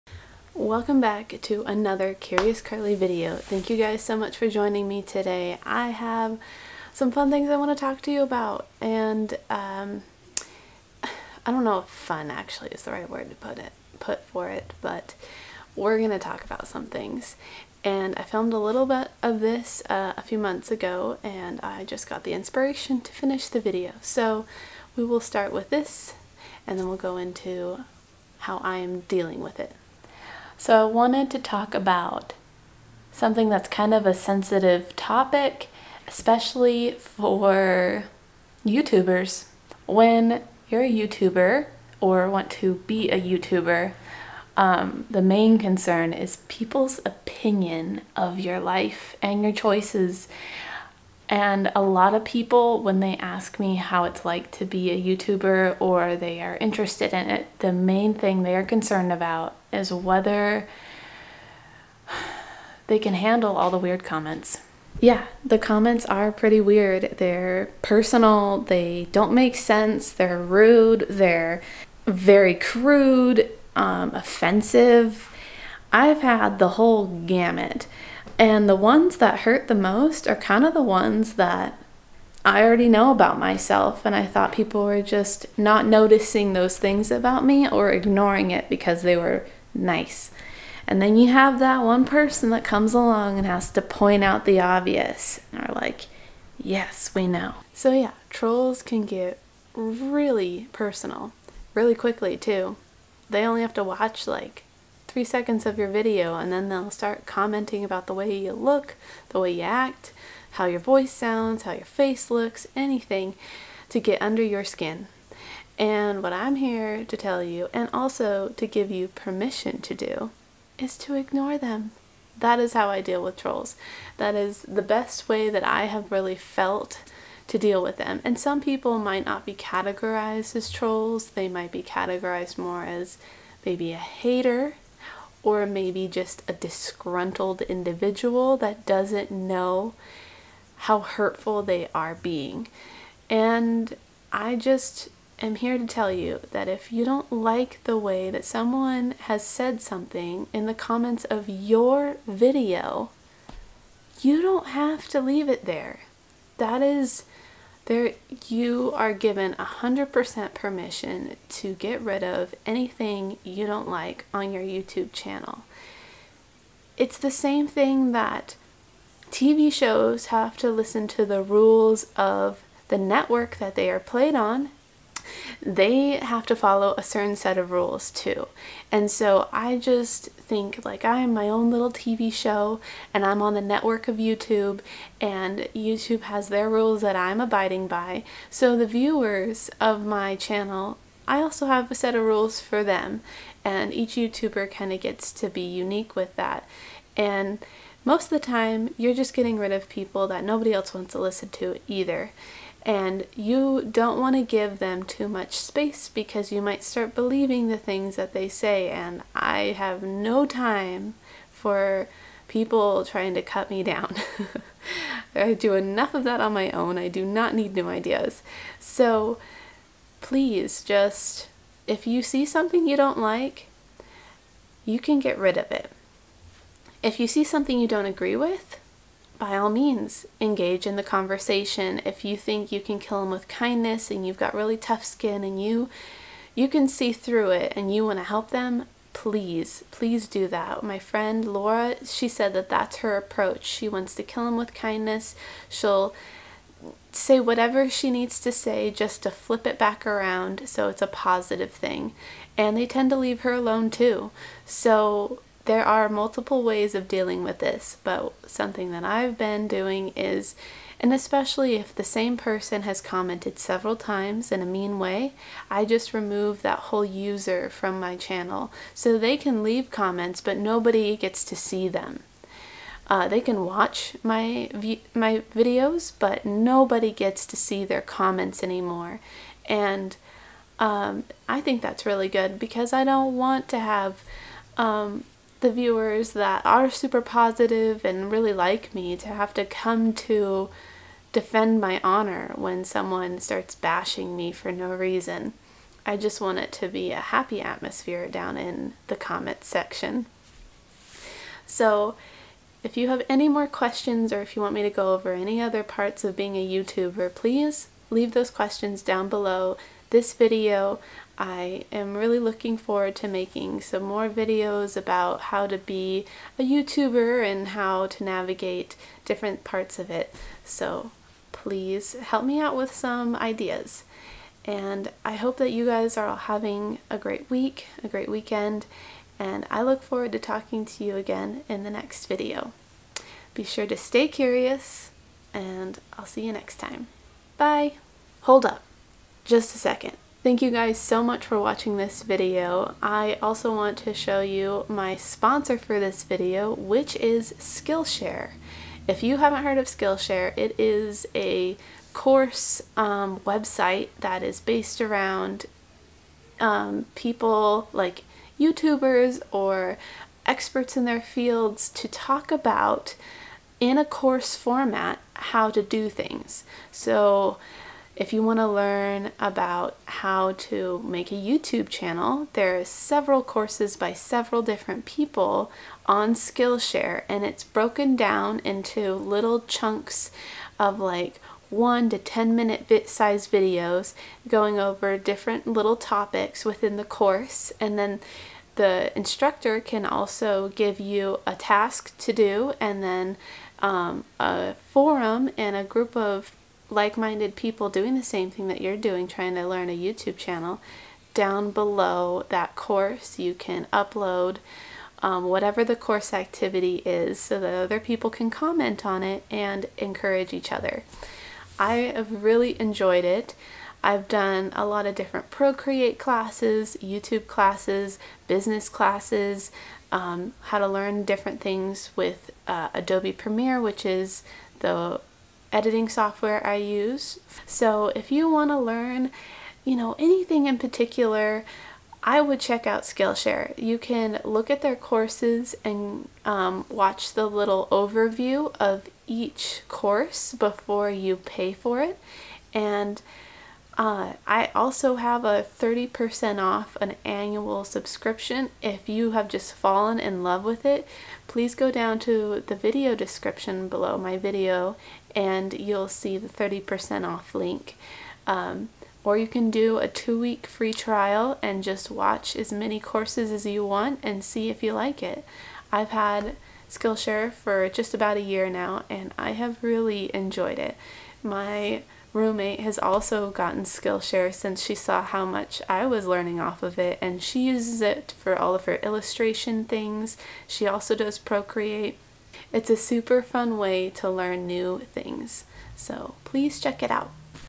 dealing with trolls
Dealing with trolls is a tricky subject. Here is how I manage it.